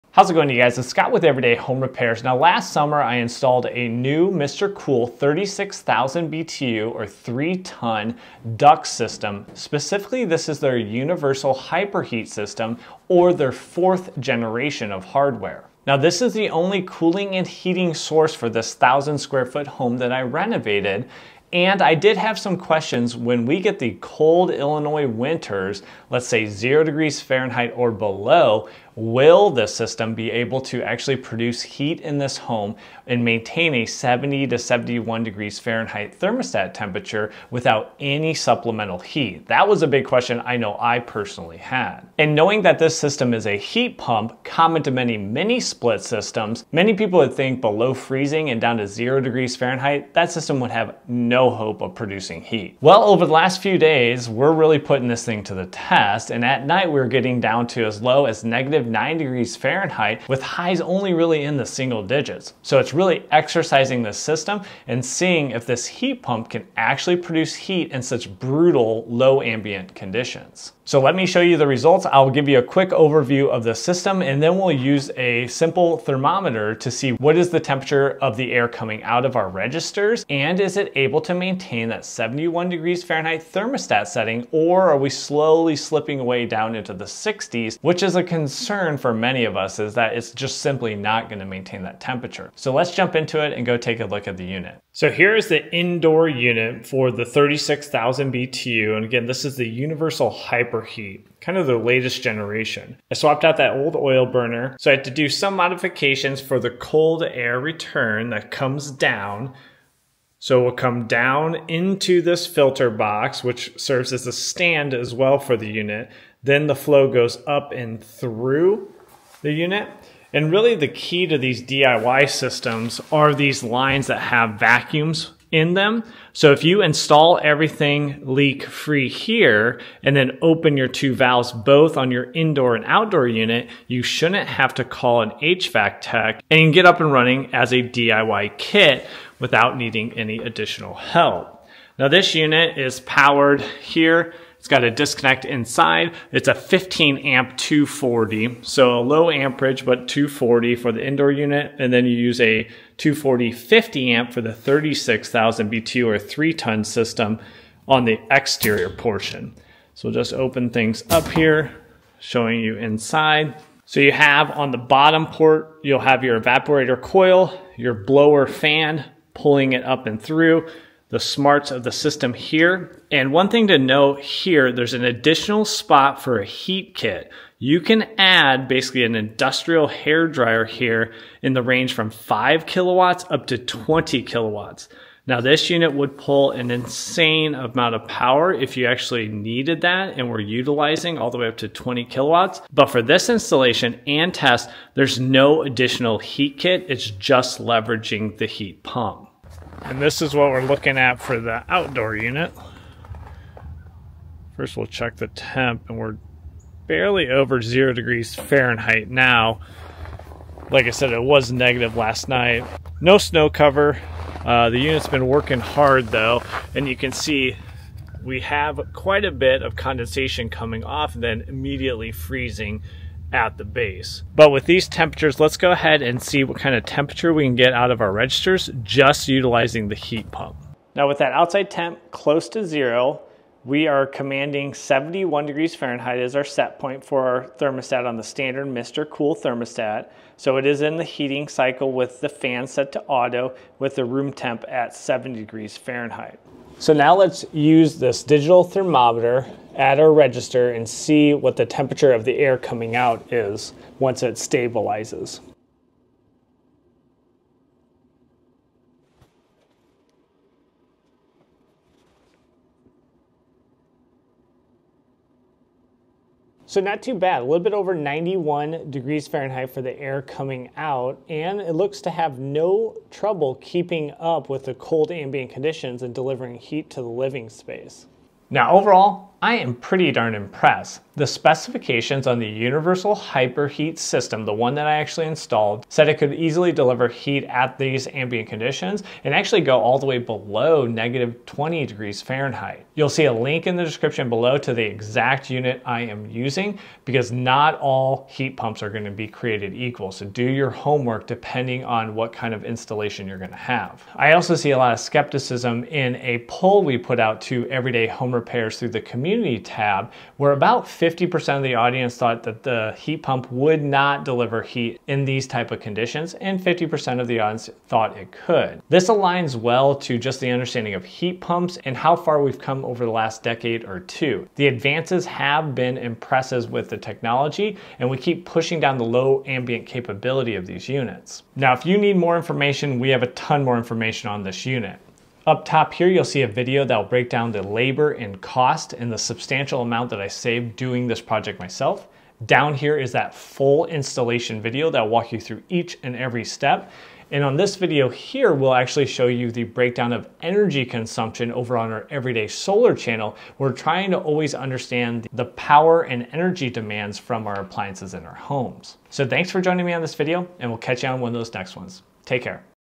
Testing A MrCool At Extreme Cold Temps | Will The Hyper Heat Work?
Mr Cool Hyper Heat Ducted (2 Ton)
Mr Cool Hyper Heat Ducted (3 Ton)
Mr Cool Hyper Heat Ducted (4 Ton)

I installed a 36,000 BTU (3 Ton) MrCool Universal Hyper Heat ducted system a few months back and I always wonder how it would perform in extreme cold.  This week we have a shot of cold air like most of the country so I took this as the opportunity to test the system's capabilities and share the results with you so you can make an informed decision if you are considering a MrCool heat pump system.

Free Home Maintenance Checklist

Friends Don't Let Friends Tape Outlets T-Shirt